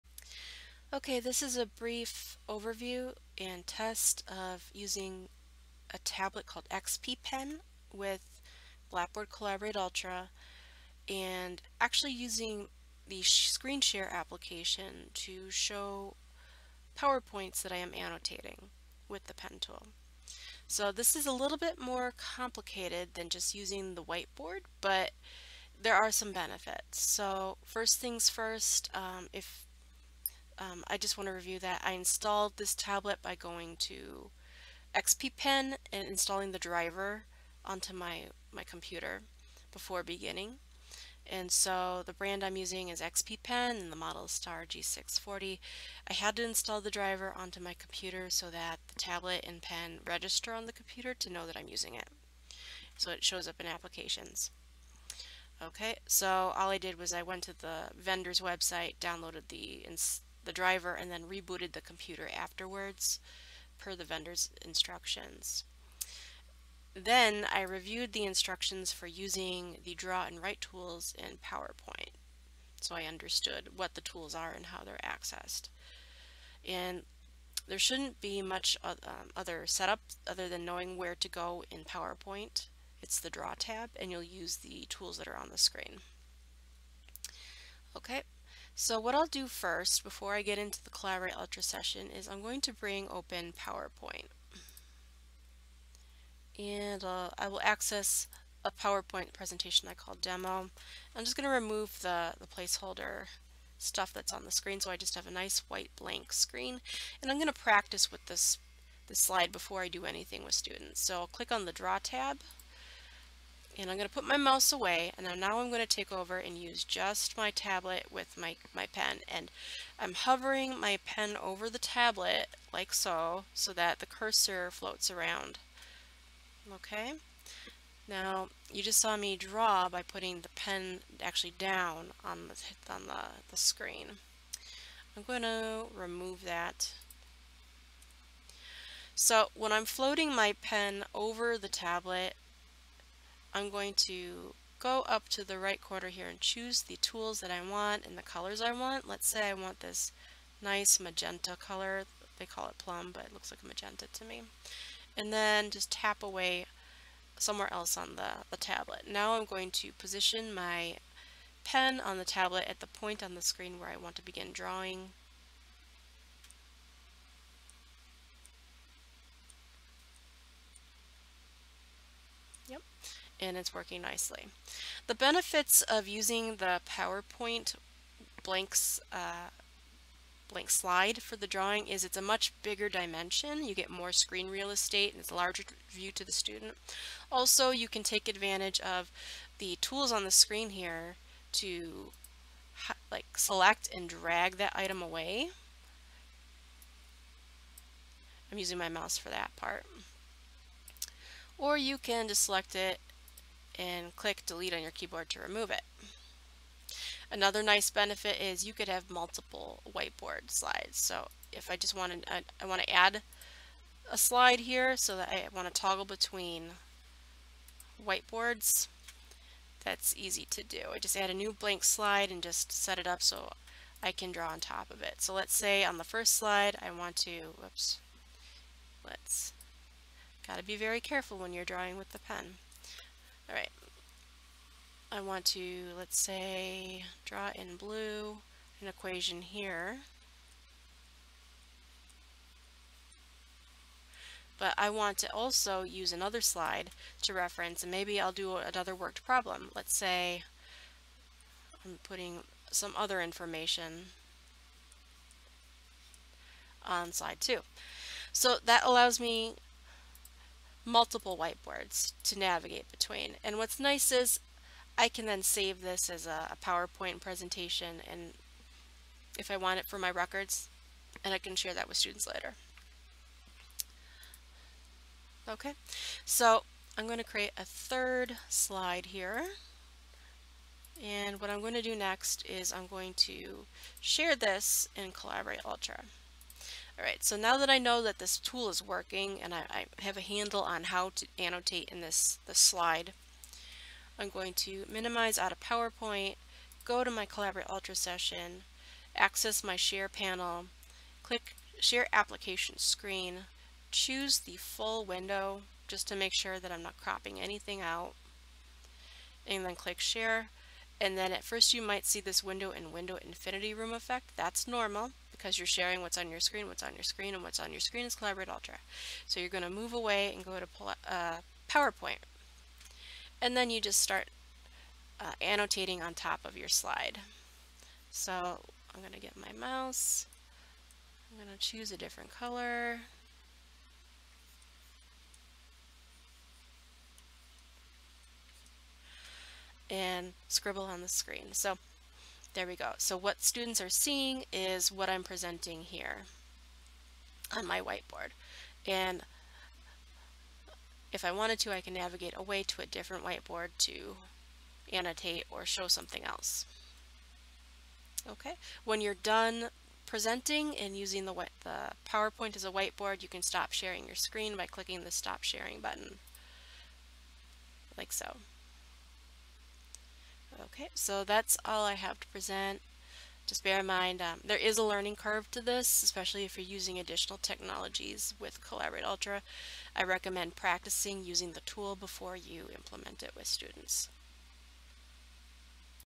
Using a Pen and Tablet with Collaborate Ultra and PowerPoint
This is a demonstration of using a pen and tablet system called XP Pen, with Collaborate Ultra to annotate blank PowerPoint slides. This is an alternative to using the Collaborate Ultra whiteboard. PowerPoint slides may provide you with a larger canvas and additional tools.